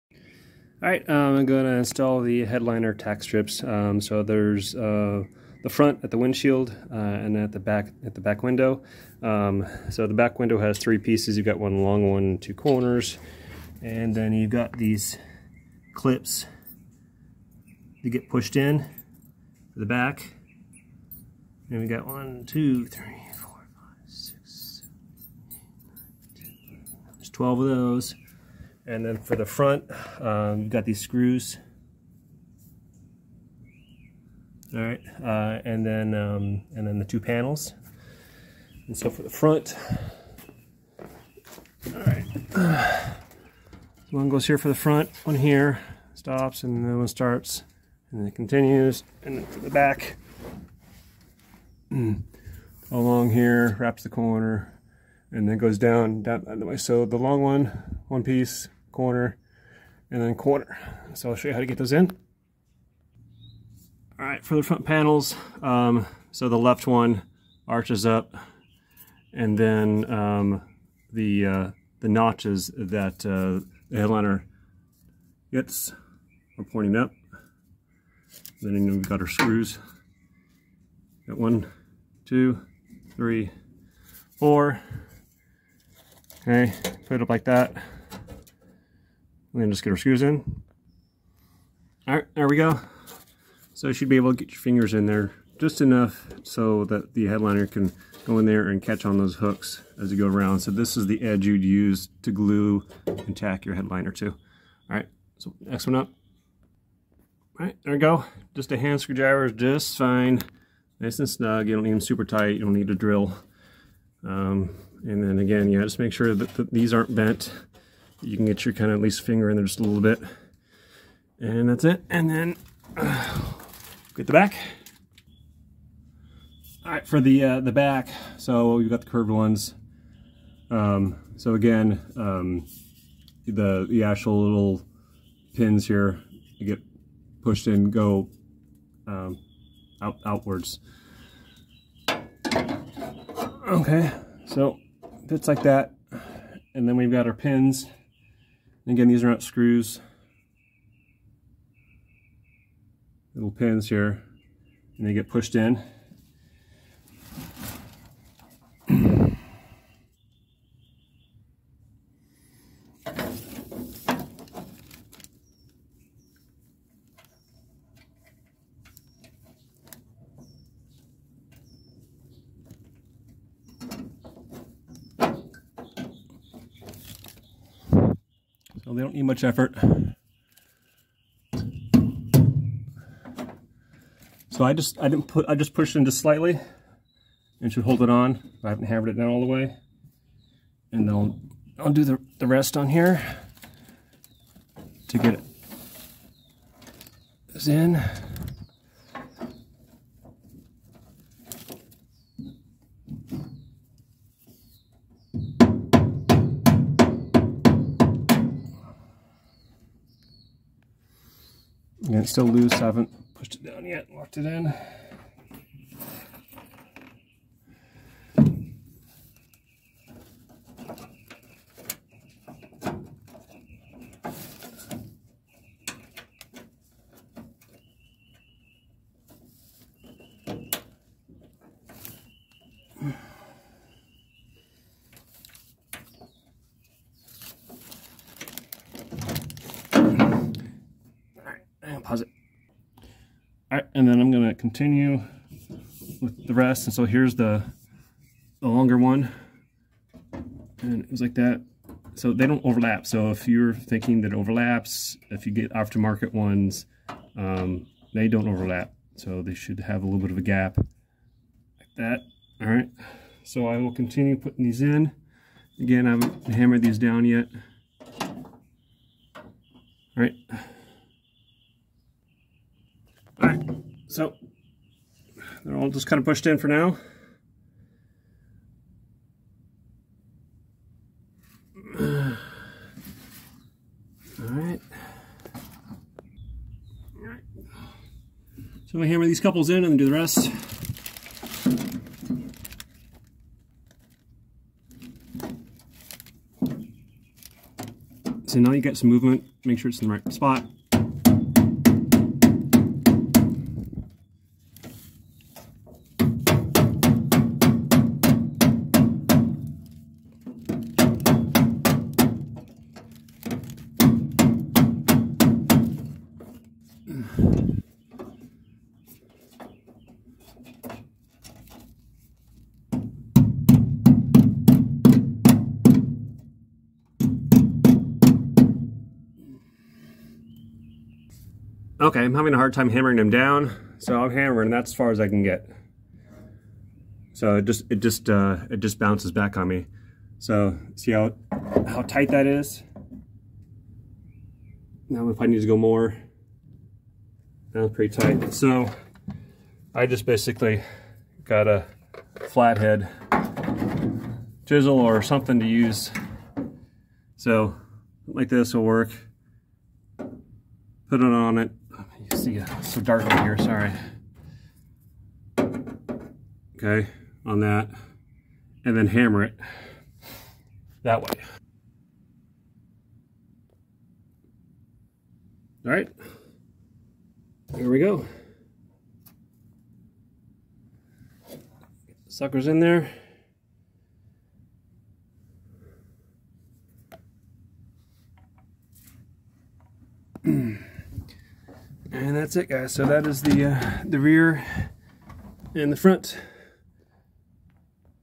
1968 Camaro Headliner Tack Strip Installation: 327
Installation of the front and rear headliner tack strips.